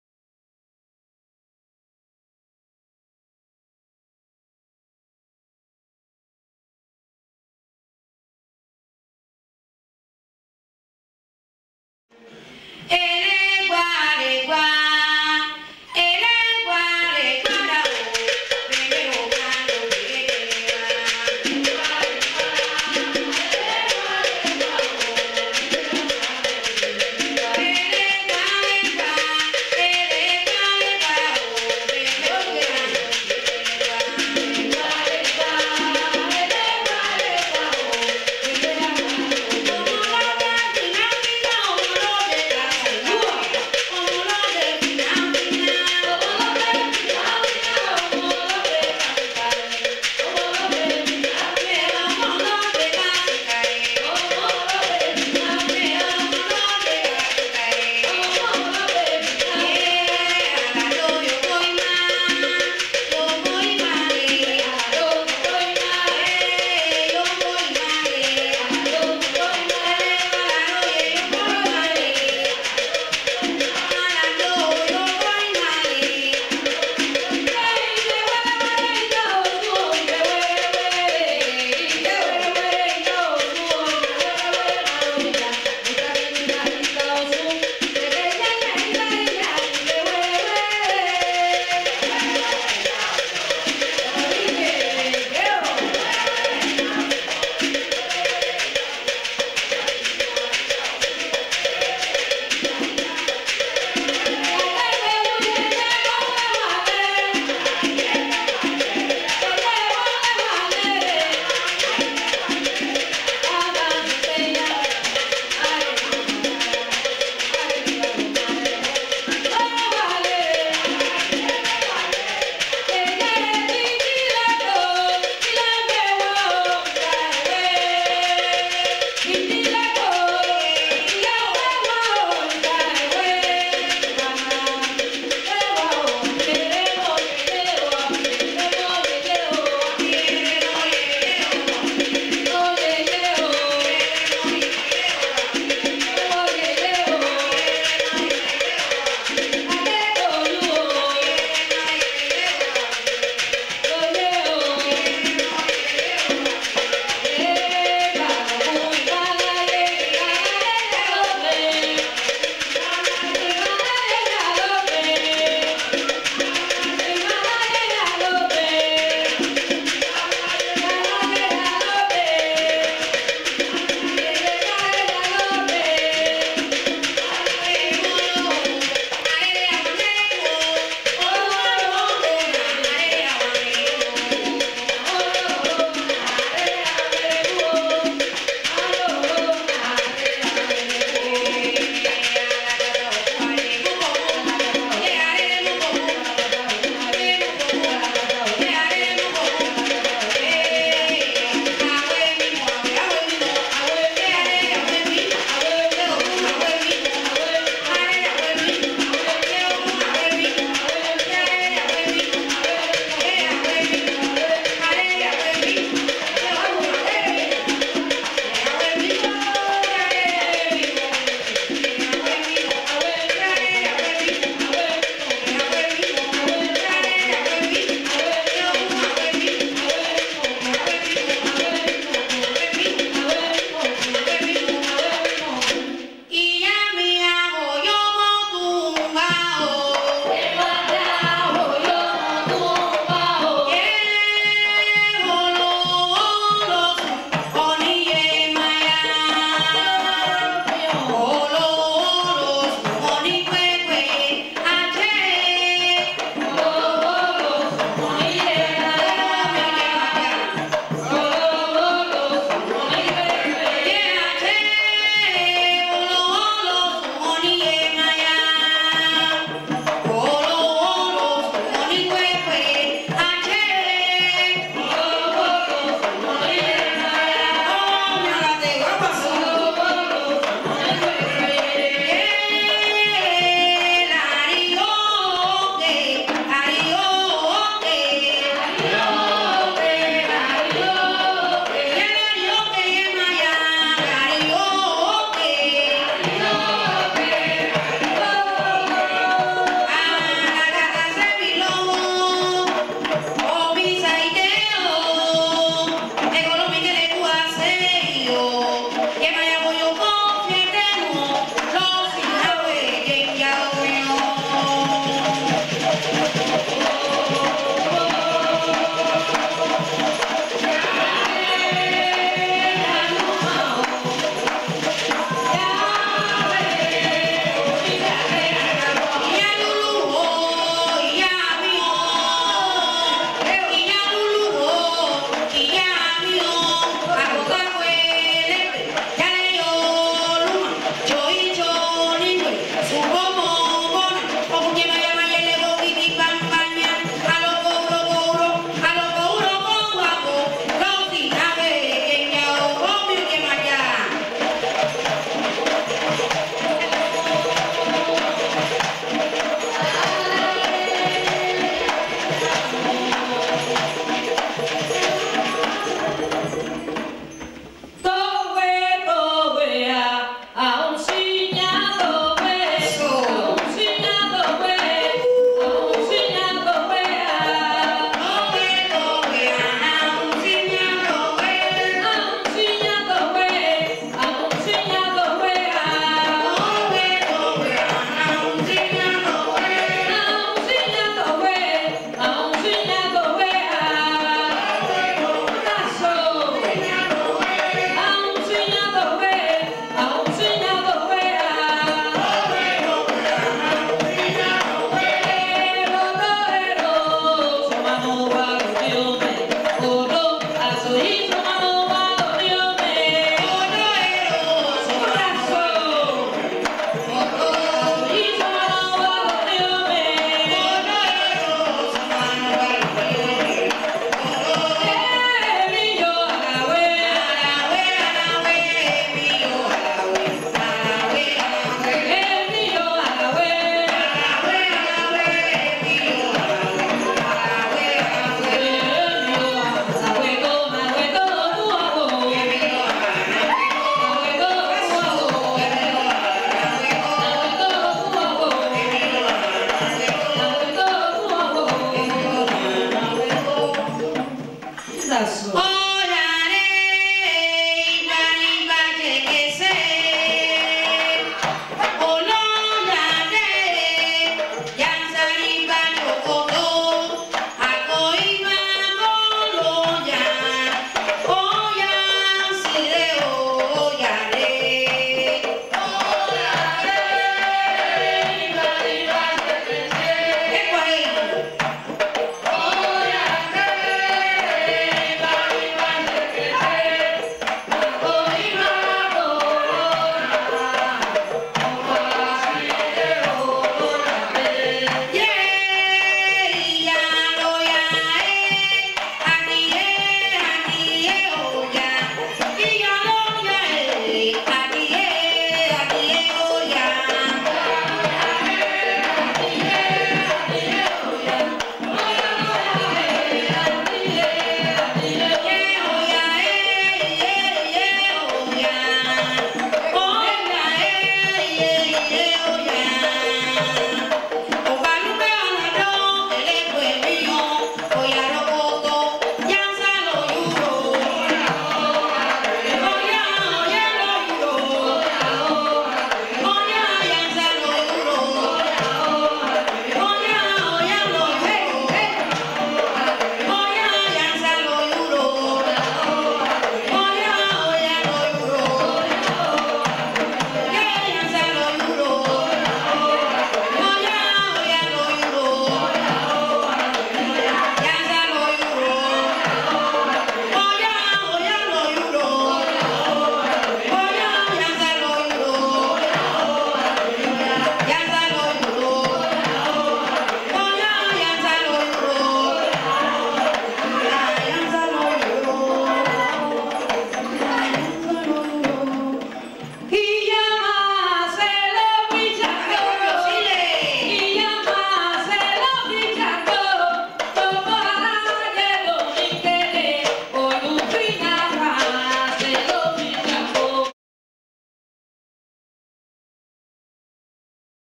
ORICHAS 1 - CFNC (Conjunto Folklorico Nacional de Cuba) 1991
Conjunto Folklorico Nacional de Cuba en France en 1991 (Noisy-Le-Sec). Tambours batas (iyá) : Alberto Villareal. Filmé par Daniel Chatelain.